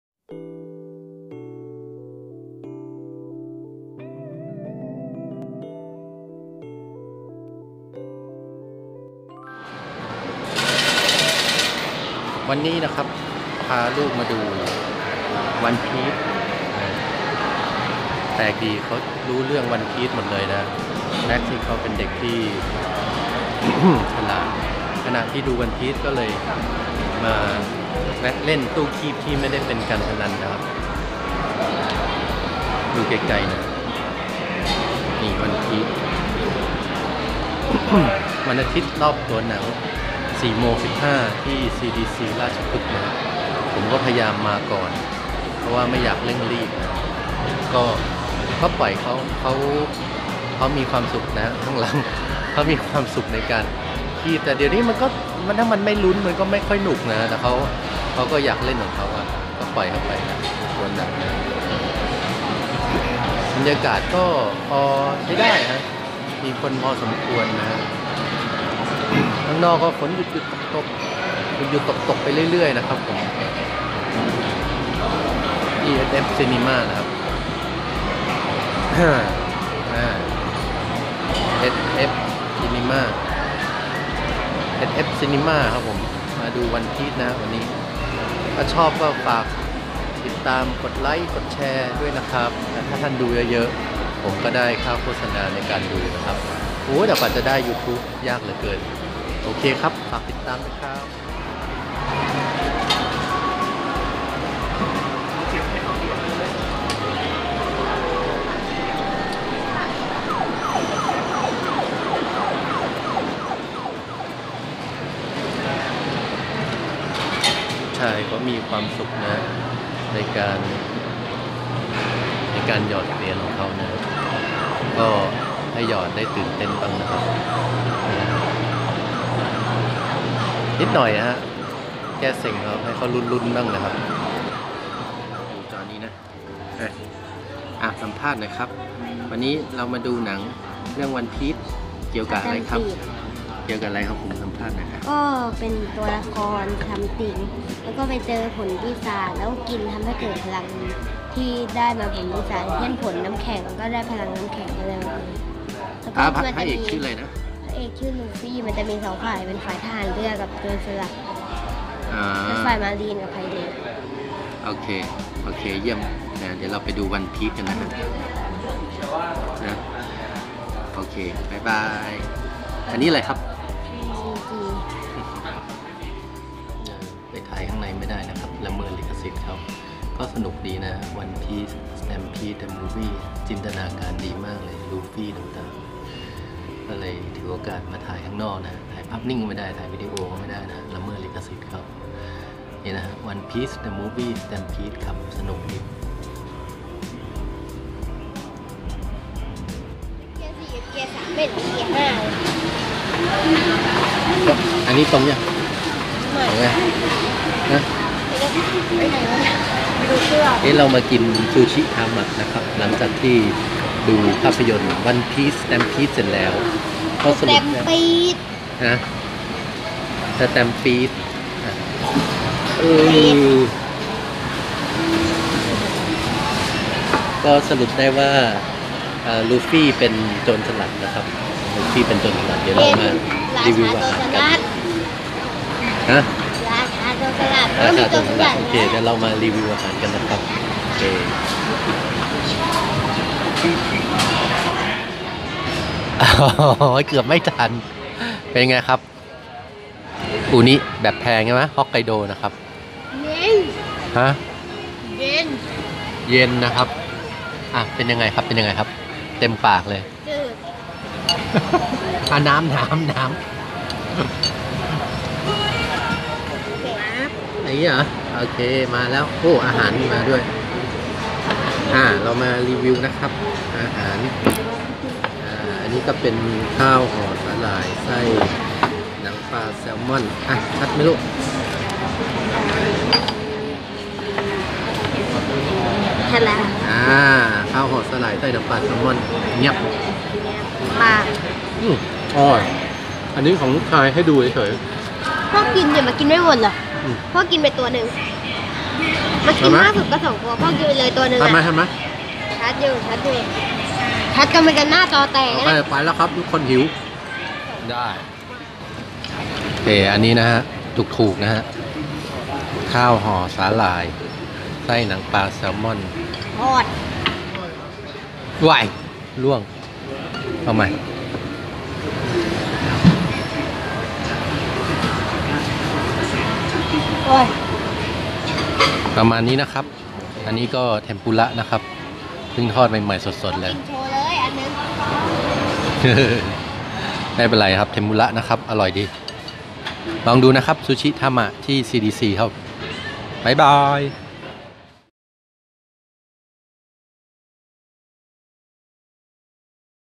ดูหนัง กินข้าว เรื่อยเปื่อย
วันนี้ Max ลูกชาย ชวนพ่อไปดู ภาพยนตร์ One Piece Stampede
ลูกไปกับพ่อ ๒คน ก็บันทึกไว้เป็นความทรงจำ
ดูหนังจบก็ไปทาน Sushi Tama ร้านประจำ ที่ CDC ถนนราชพฤกษ์ครับ
ดูเรื่อยๆ ไม่เร่งรีบนะครับ
ไม่มีผลประโยชน์ทางการค้า ทำคลิปเองทั้งหมด จ่ายเงินเอง